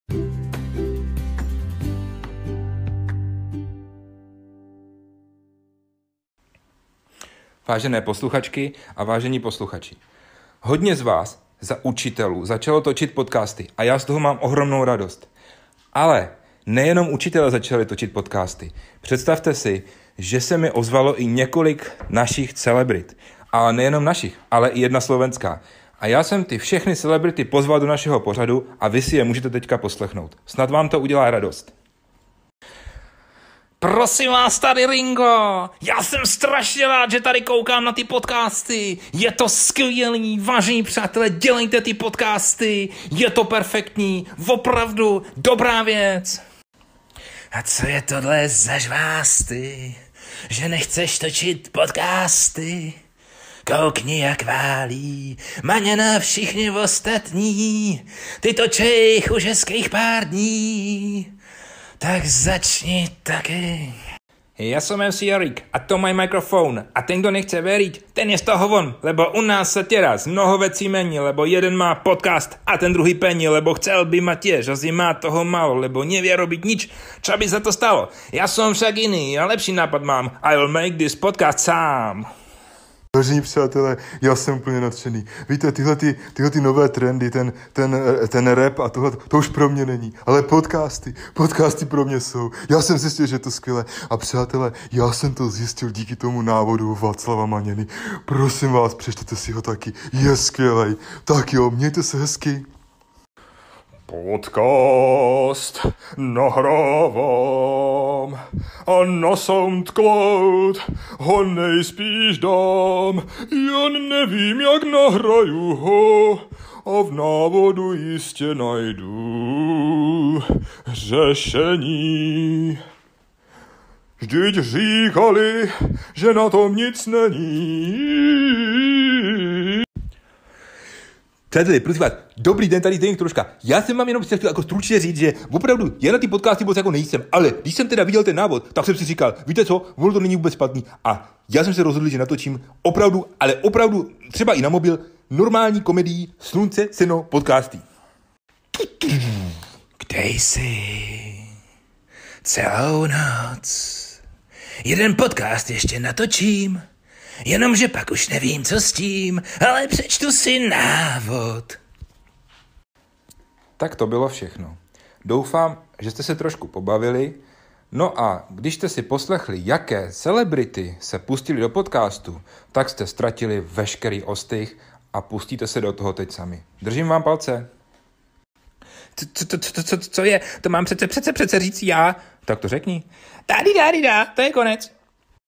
Poděkování učitelům a ukázky podcastů slavných lidí
Tento podcast vznikl jako poděkování všem, kteří našli odvahu začít.